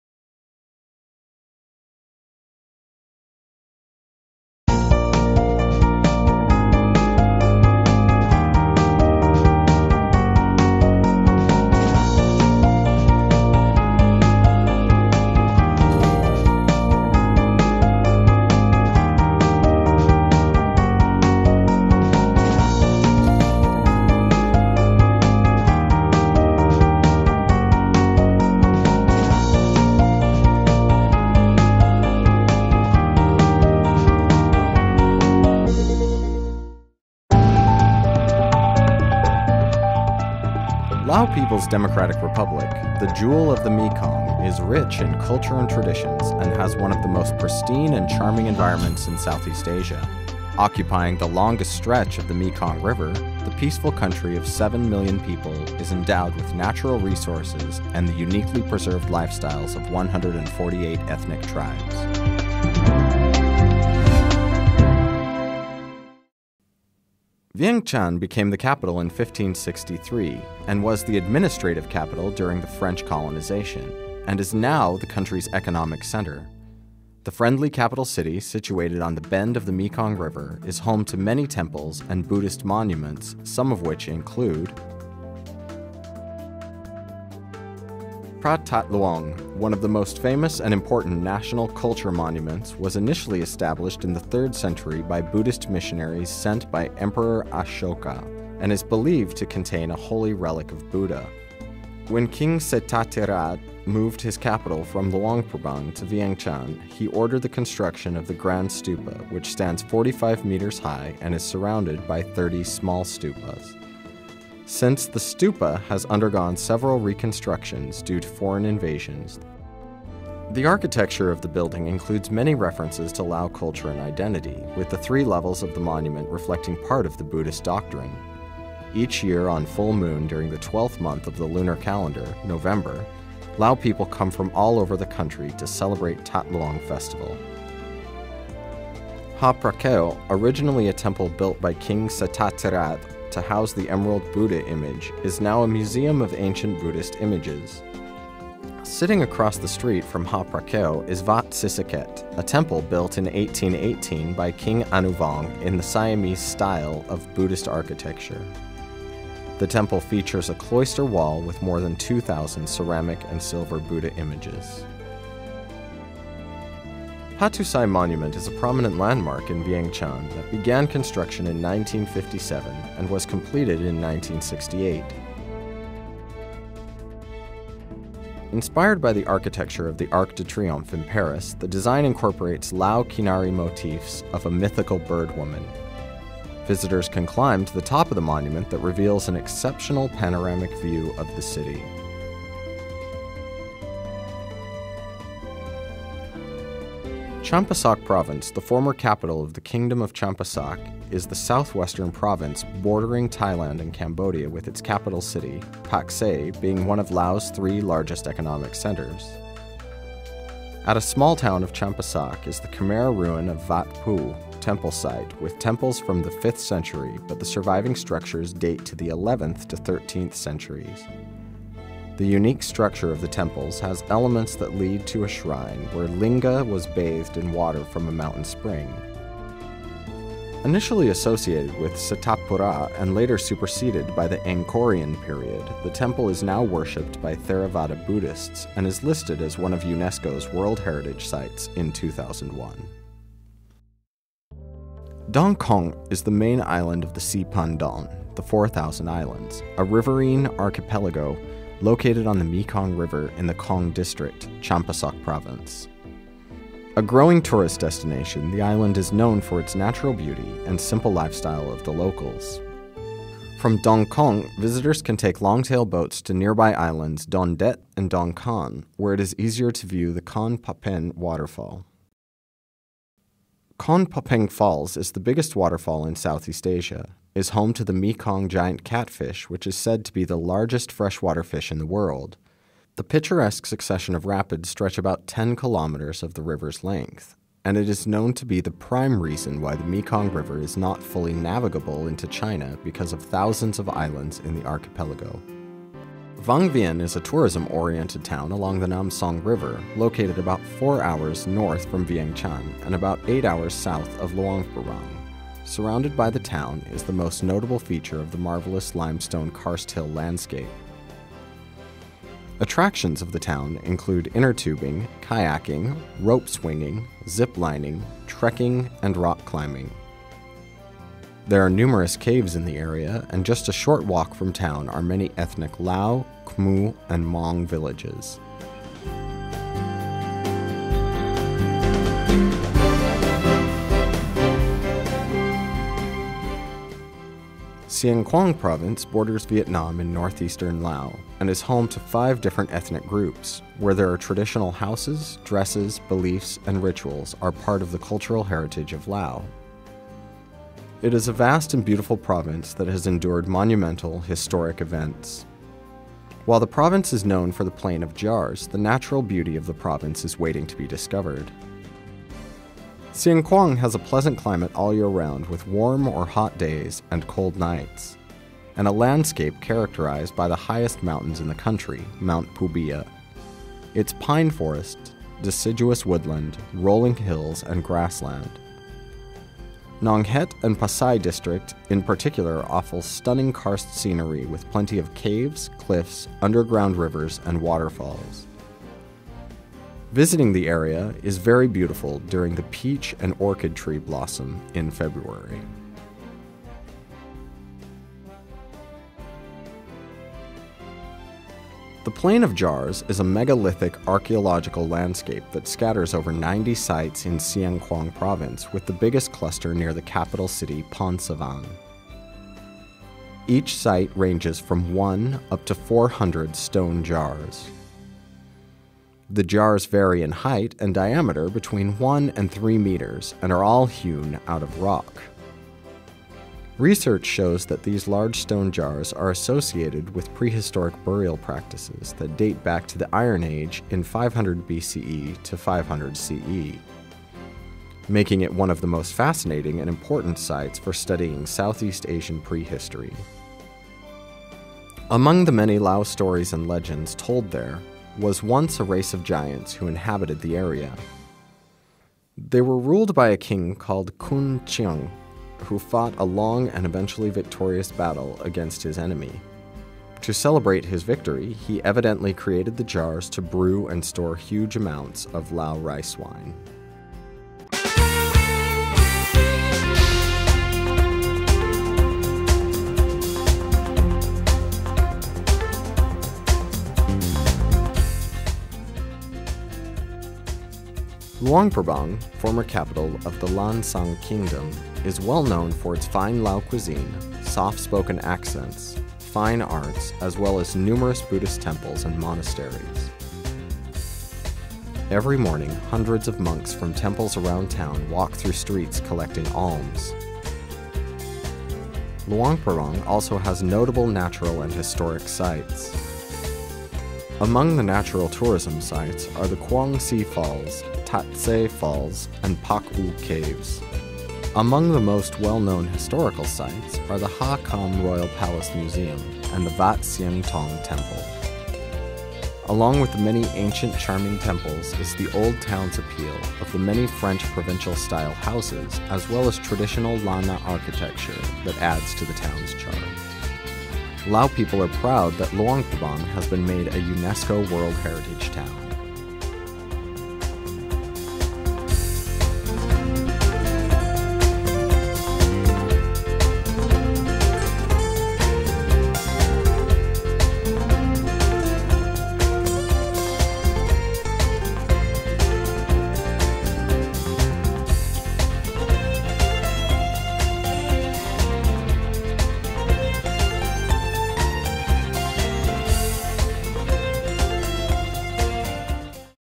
Lao People's Democratic Republic, the jewel of the Mekong
Lao People's Democratic Republic, the jewel of the Mekong is rich in cultural traditions and has one of the most pristine and charming environment in Southeast Asia.